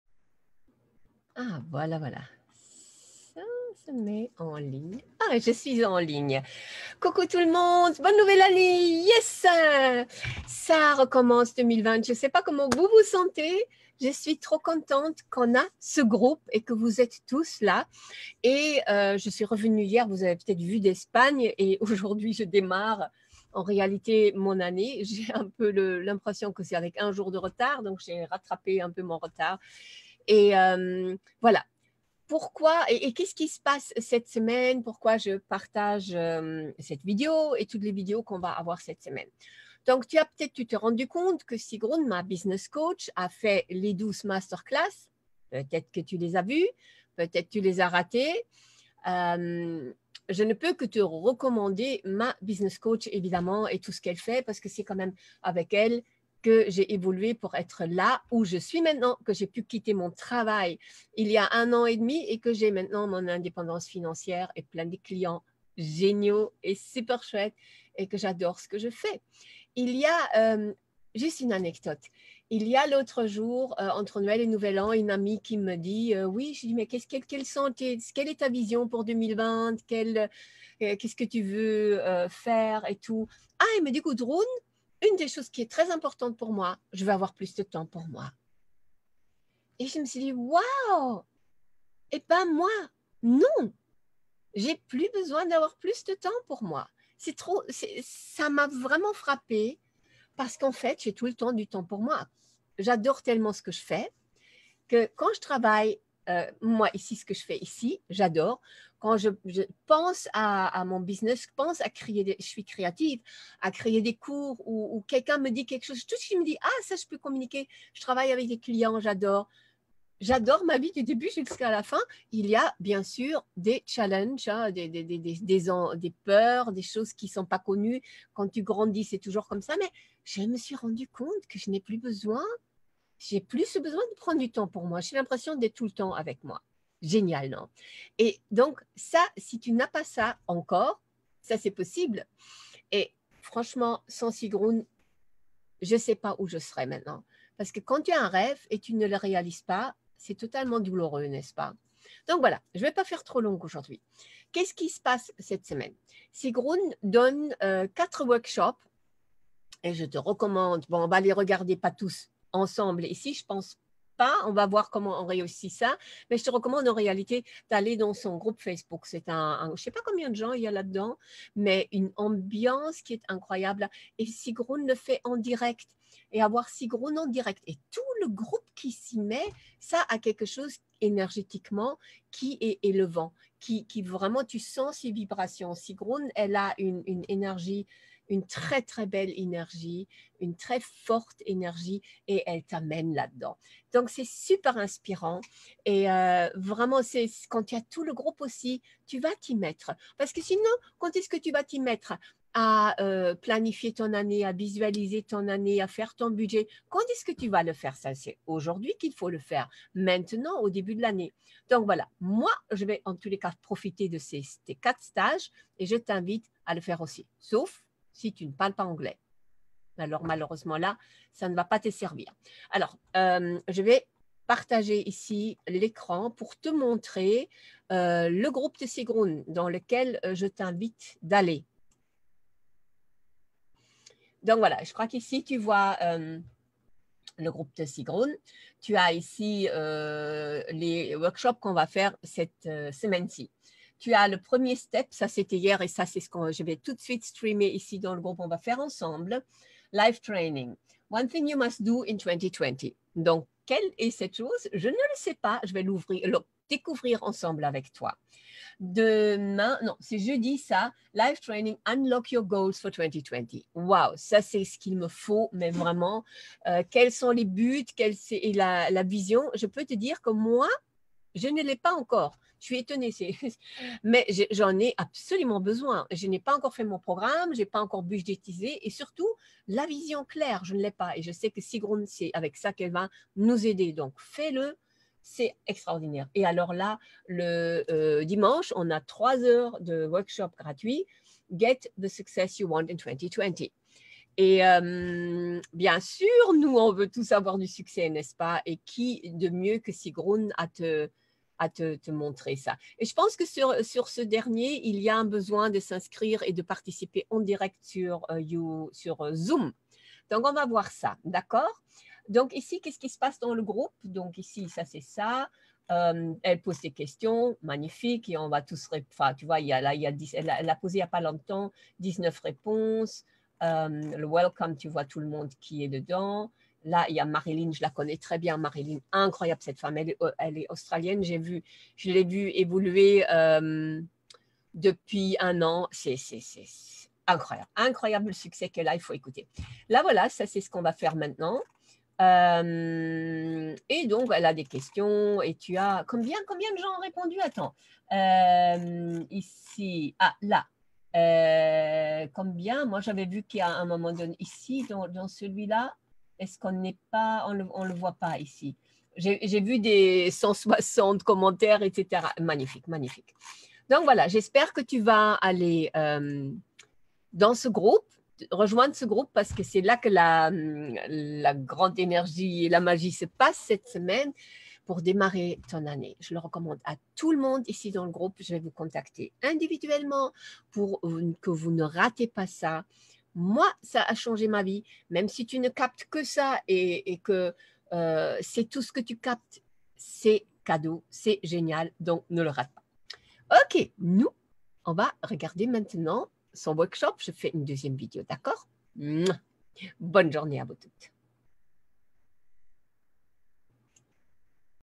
J'explique les 4 workshops avec ma business coach, Sigrun, cette semaine
Je te recommande vivement de les suivre. J'explique ici pourquoi.
Rejoins mon groupe "Porteuse de projet", une sororité d'entretaide et de business women